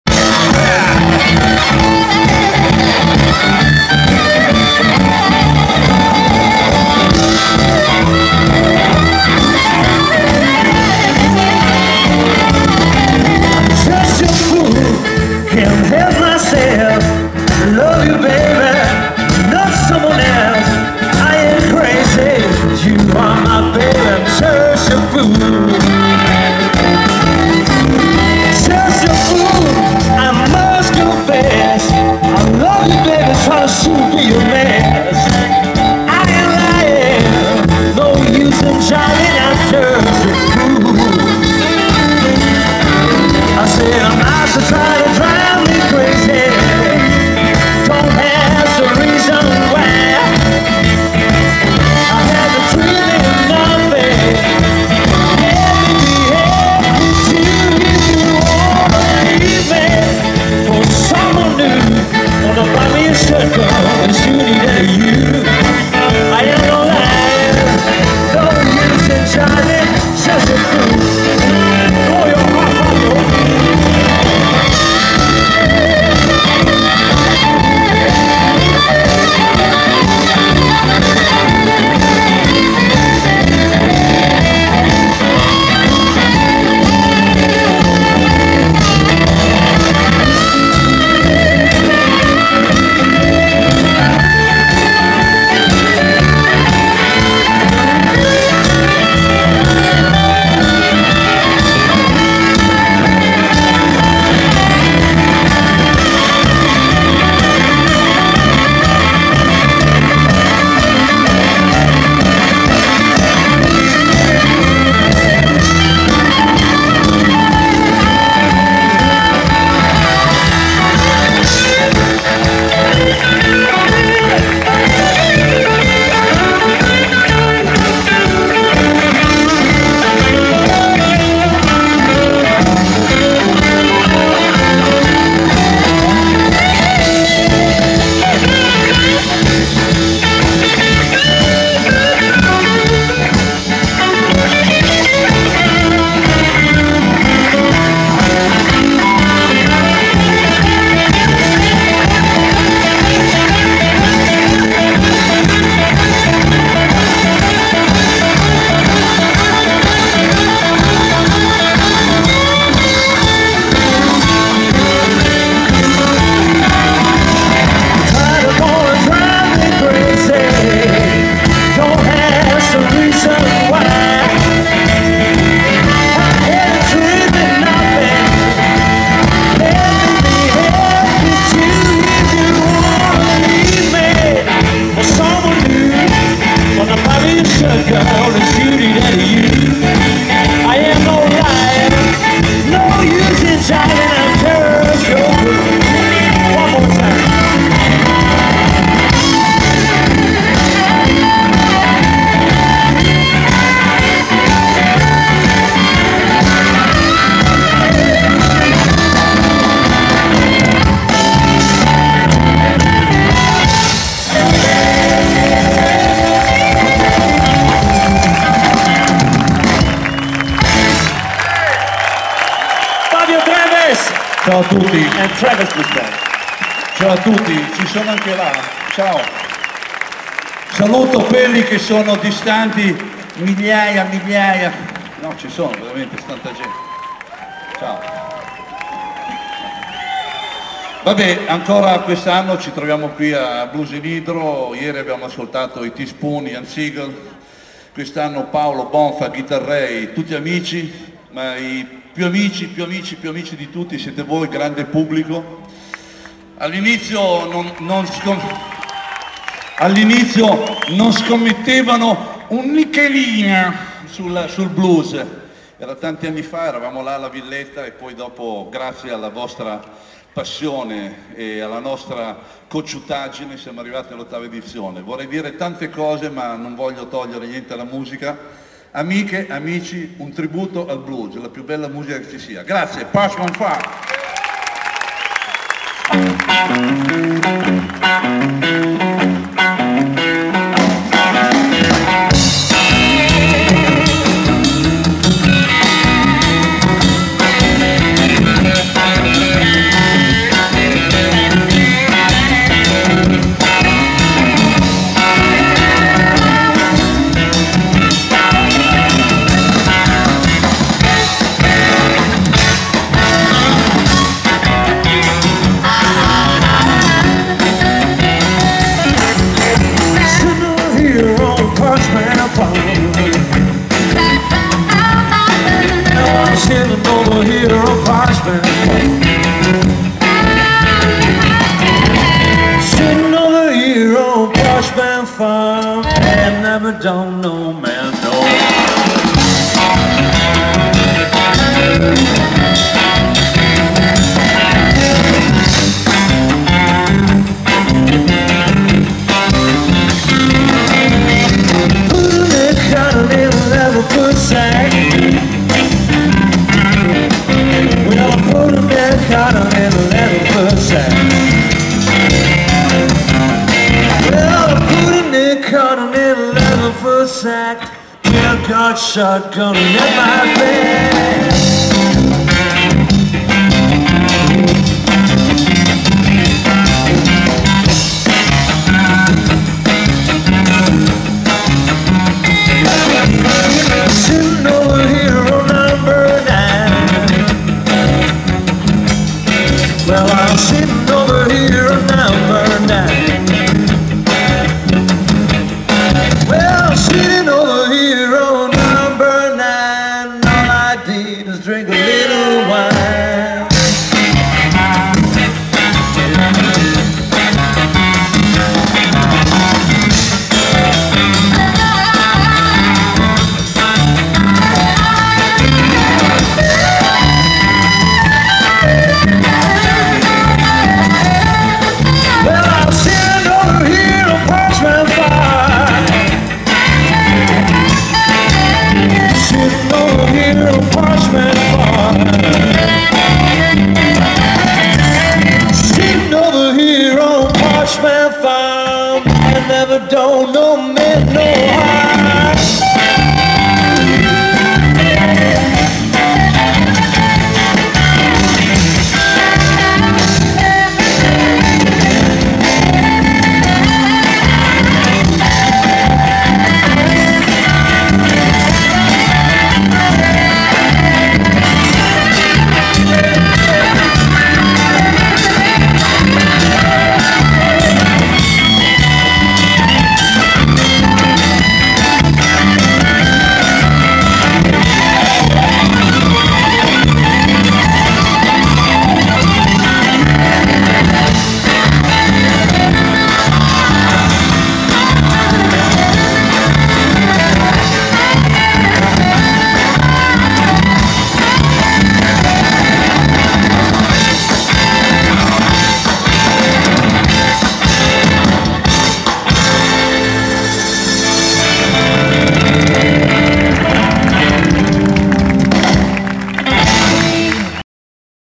Treves Blues Band Blues in Idro 2012
Malgrado la diaspora dei bassisti, qualche disfattista e qualche scettico che lo davano per uno che non ha più niente da dire, il Puma di Lambrate è tornato a far ruggire la sua armonica nella notte milanese di Blues in Idro 2012, appuntamento annuale che è un must oramai. Quale migliore risposta a chi storce la bocca?
Grande Puma! G.G.King